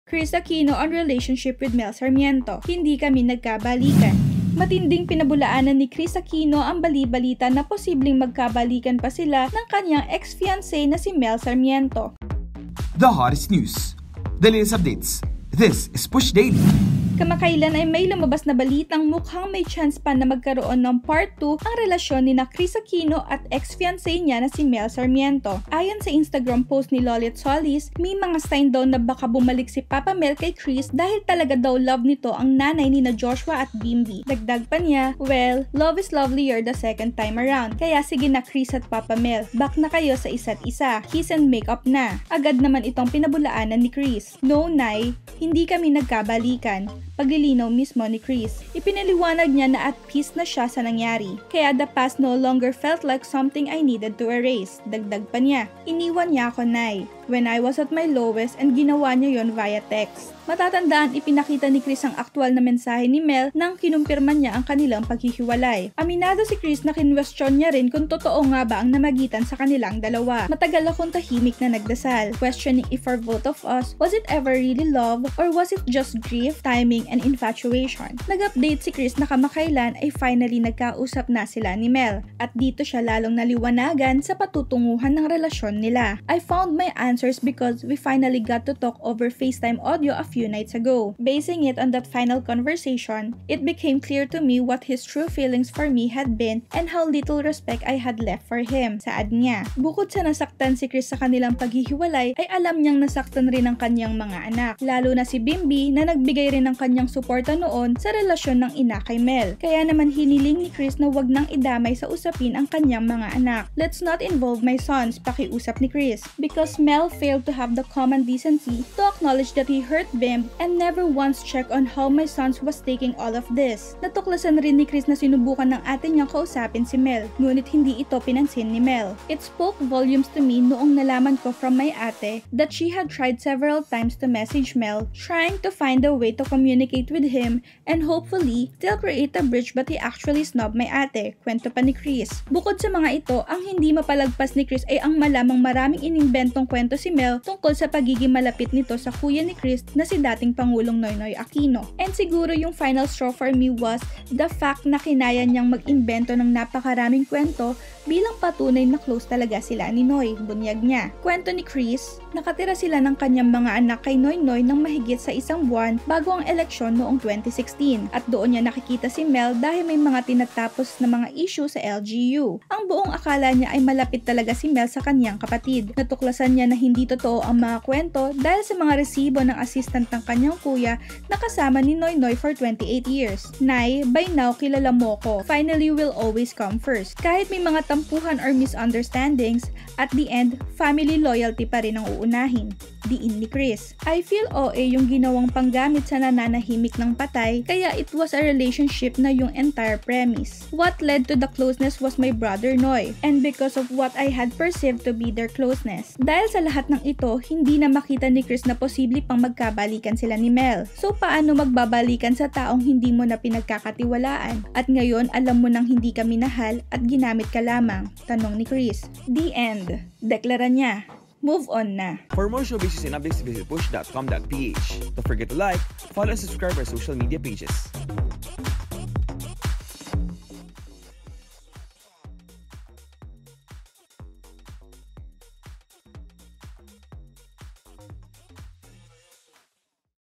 Kris Aquino on relationship with Mel Sarmiento: ‘Hindi kami nagkabalikan’ | PUSH Daily
PUSHDaily: Matinding pinabulaanan ni Kris Aquino ang bali-balita na posibleng magkabalikan pa sila ng kaniyang ex-fiancé na si Mel Sarmiento. Full story on push.com.ph!